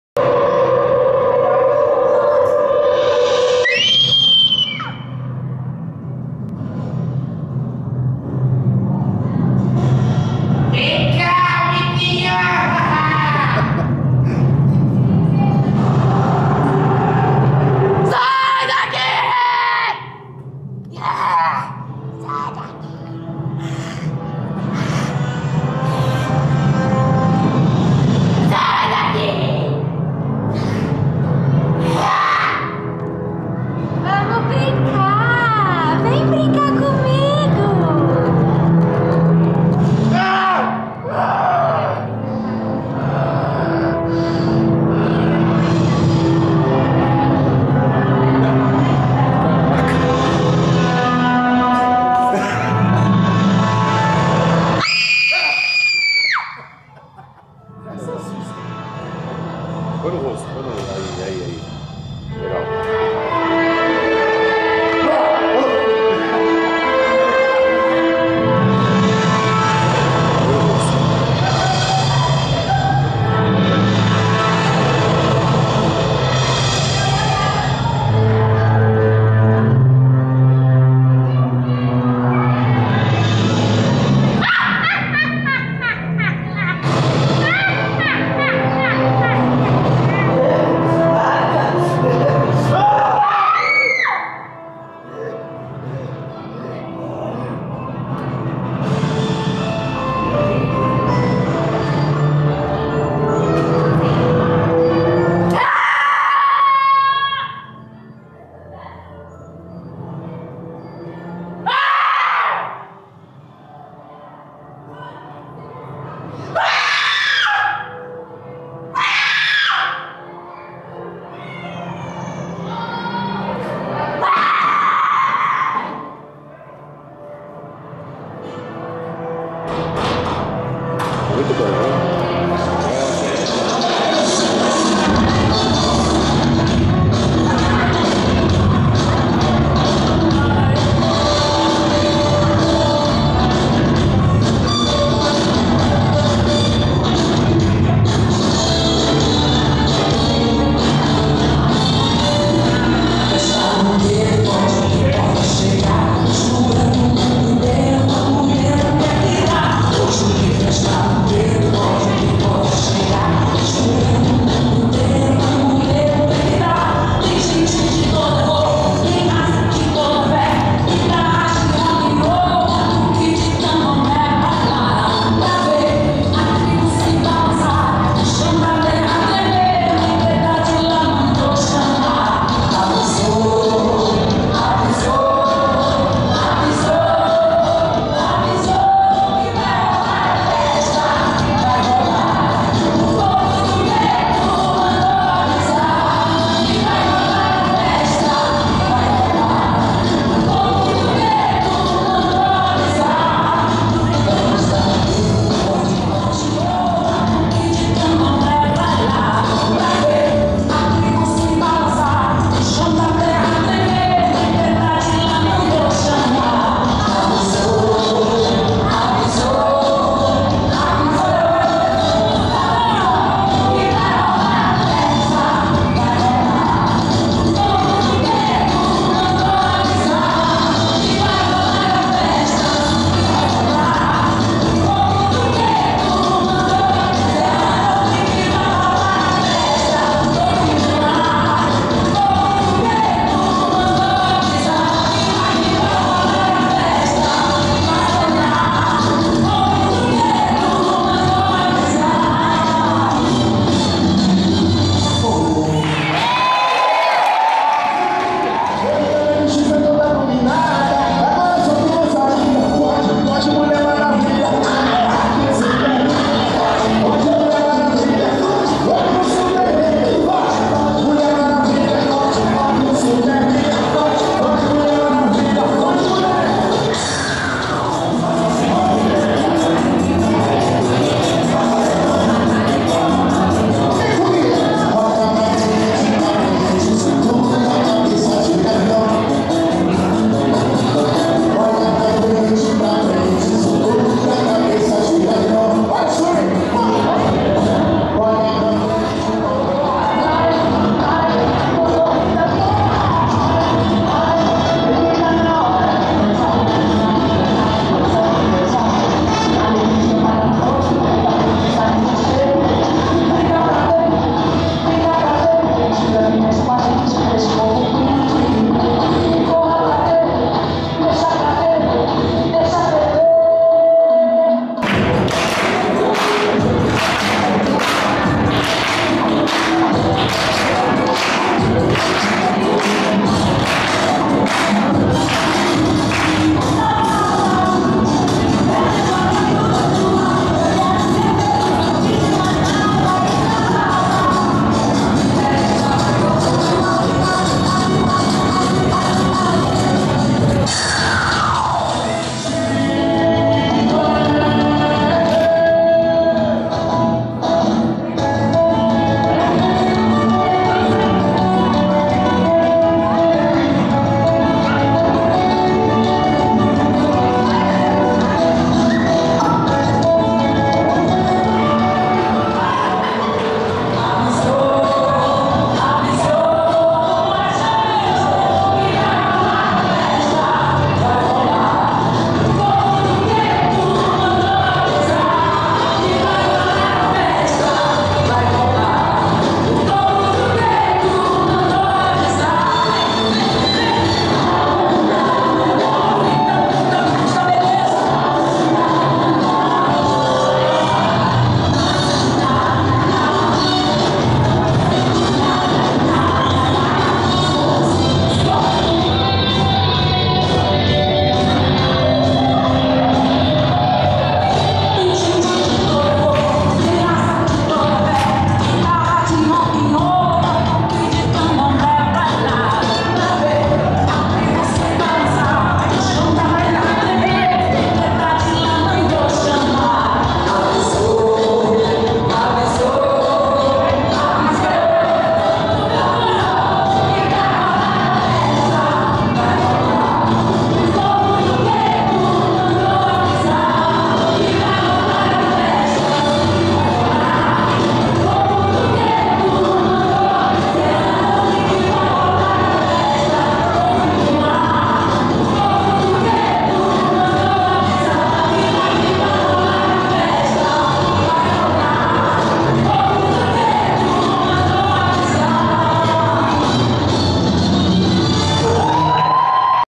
Festa da Primavera 28/09/2013
Apresentação da casa do medo e danças da catequese e perseverança em 28 de setembro de 2013.